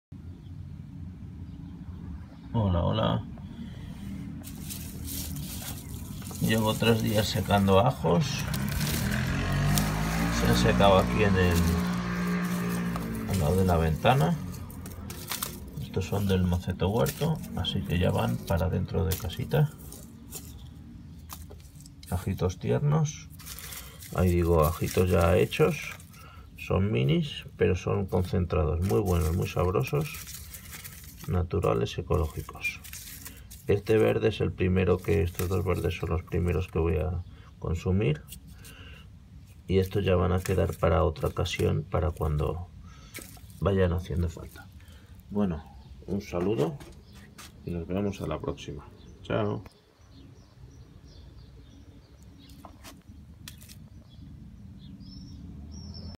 Secando ajos en cabeza
Elección de los mejores ajos para sembrar .

    Siembra de los ajos en macetasde barro manteniendo la distancia de siembra de 5 dedos

    Cuidados y Riegos de los ajos, como quitar hierbas competidoras, regar solo en casos necesarios,

  Cosecha : al final del cultivo y siempre ideal en días soleados cosecharemos los ajos y los dejaremos secar en una ventana soleada o terraza 2 días o 3  luego listos para guardar en un cestito de mimbre aireado en el salón de casa.